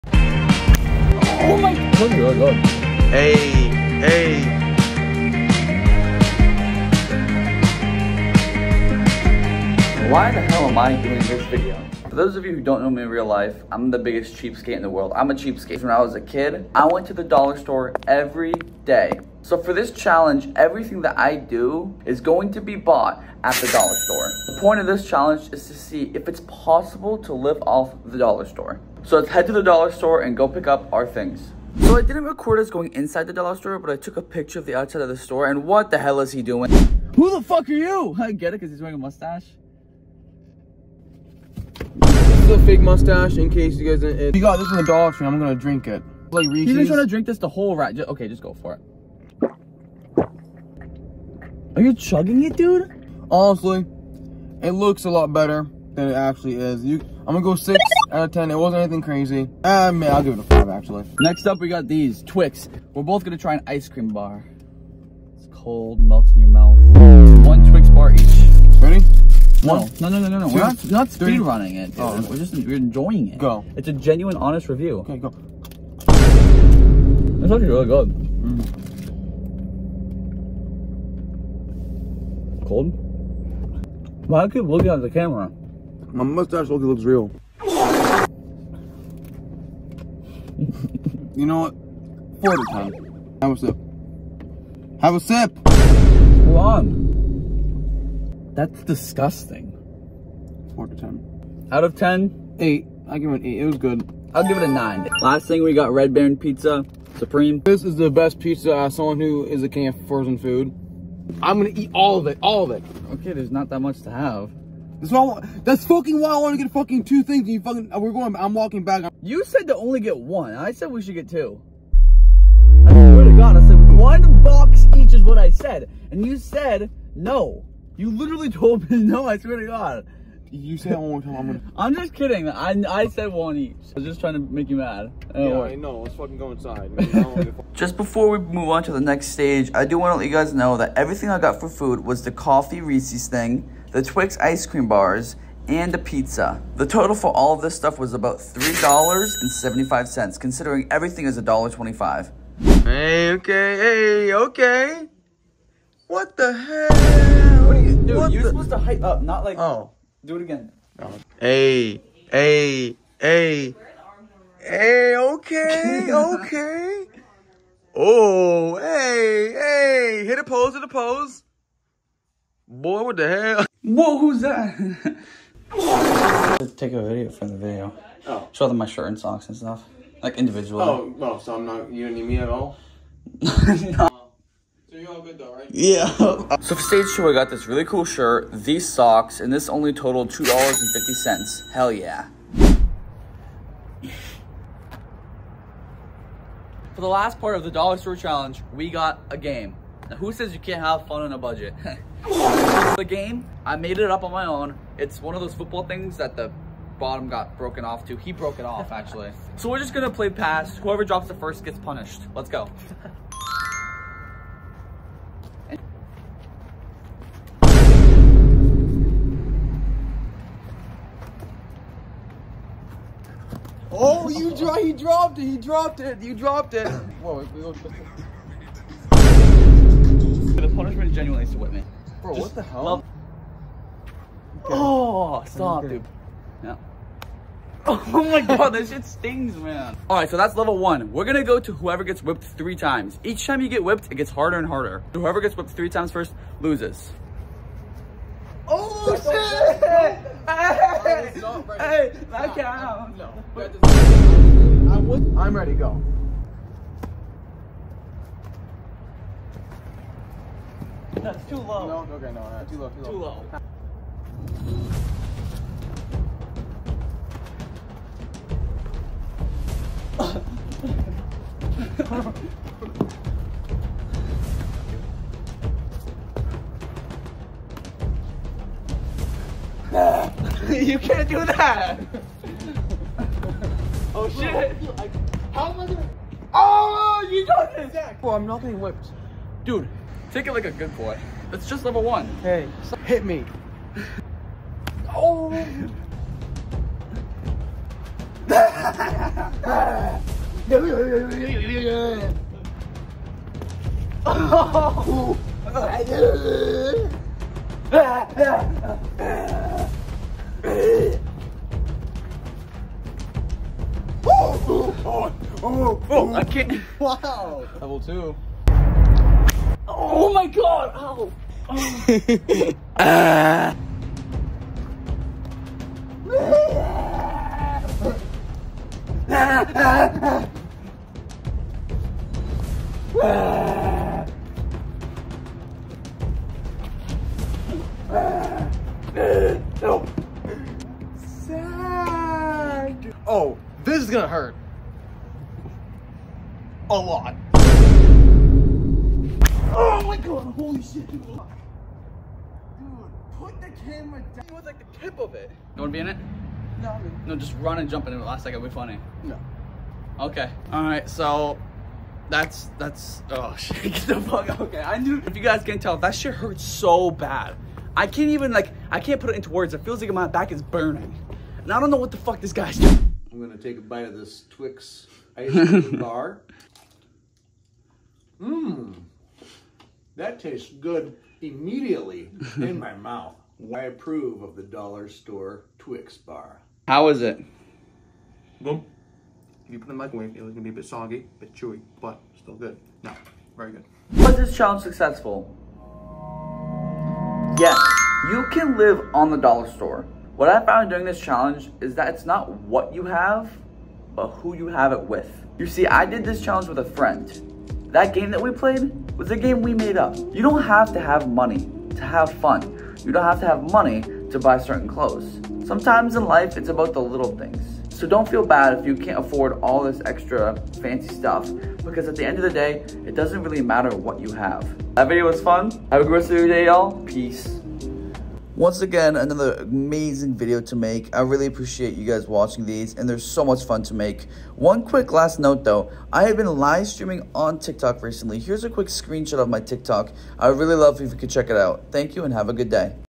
Can You SURVIVE on Just DOLLAR STORE Items?
In this video I test out how far you can really go by just using dollar store items. This doesn’t mean just food, but also clothes and activities/entertainment. Can you survive on just dollar store items? How realistic is this challenge? Find out in this video.

Stages of this video include
1. Food and Drink
2. Clothes
3. Entertainment

Check out my friend who was featured in this video at @Shadymadnes. Because he’s a big help and I feel you would like his content as well.